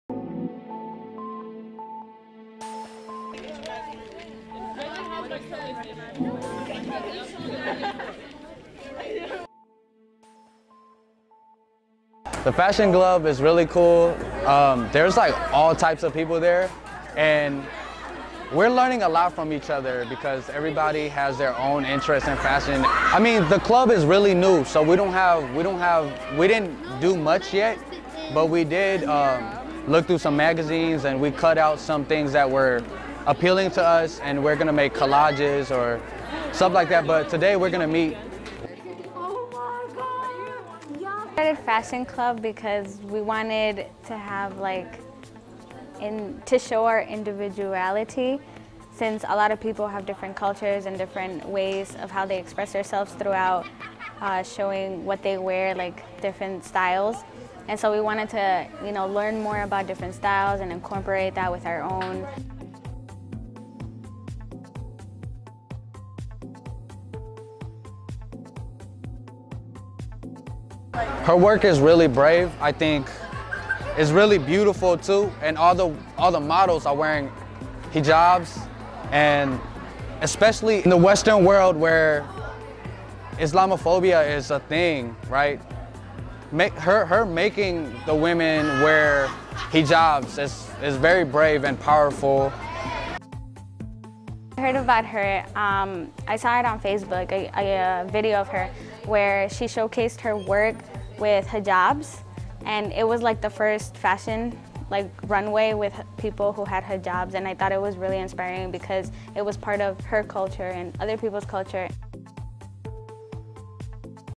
Senn High School's New Fashion Club Suits Up
Senn TV's very own Maryan Abdi reports on the new fashion club at Senn, and the inspiration they find in innovative cultural designs. Put on your best threads and enjoy this quality piece!